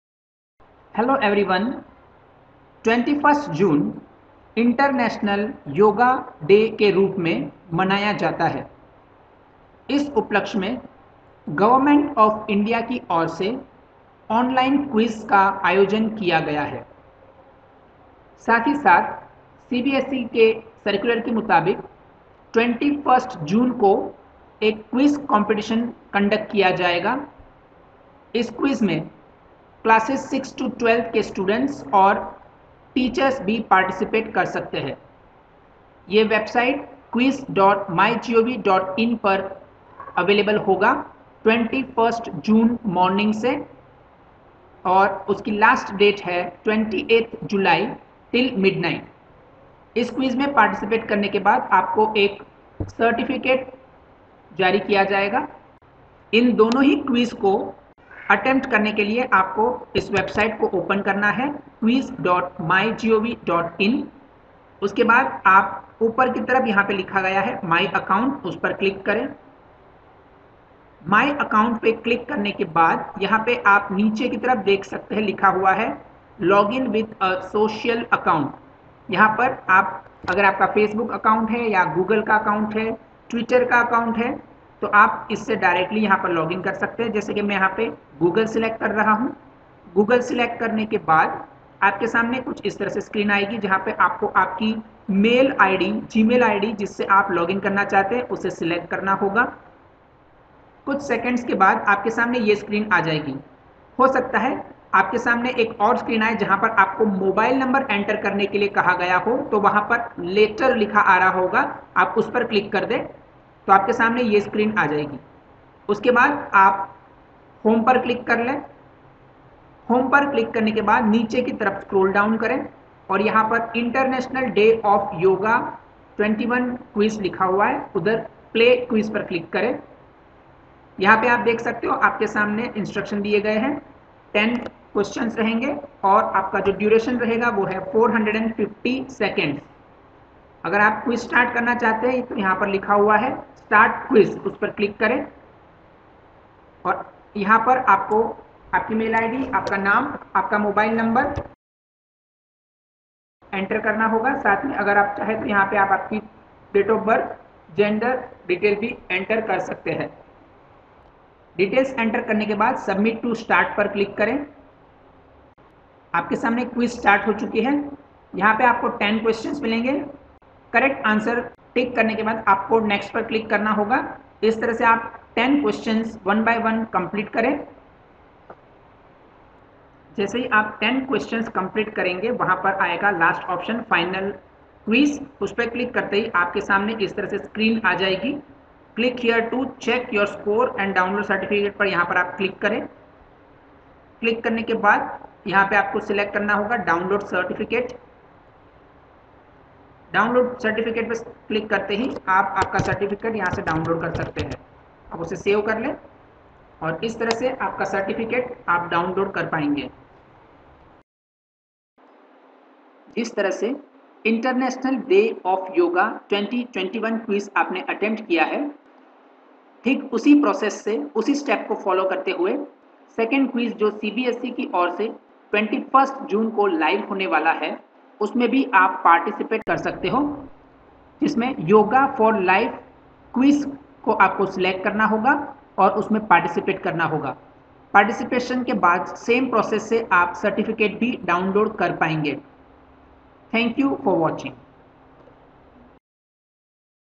How to Participate in Yoga Quiz organized by MYGOV.IN and download certificate.
INTERNATIONAL DAY OF YOGA 2021 QUIZ For Students of Classes VI to XII and for Teachers

How to Participate in INTERNATIONAL DAY OF YOGA QUIZ organized by MYGOV.IN and download E-Certificate.